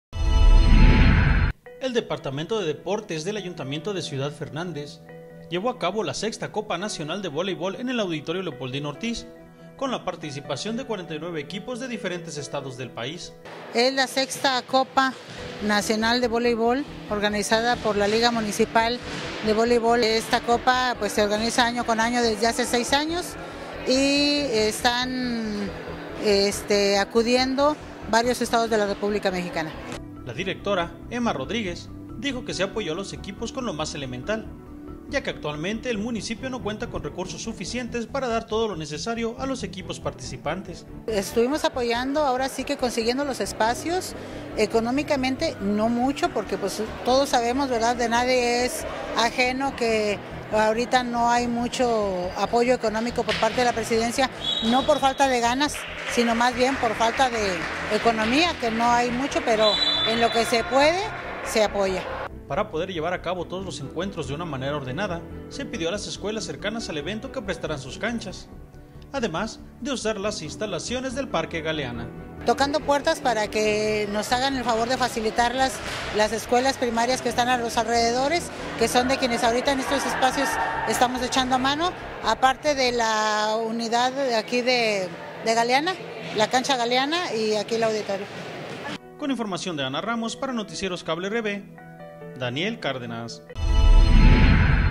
SE LLEVO A CABO LA CUARTA COPA NACIONAL DE VOLEIBOL EN CIUDAD FERNANDEZ
Con la prescencia de 49 equipos de todo el país se llevo a cabo la cuarta copa nacional de voleibol en Ciudad Fernández.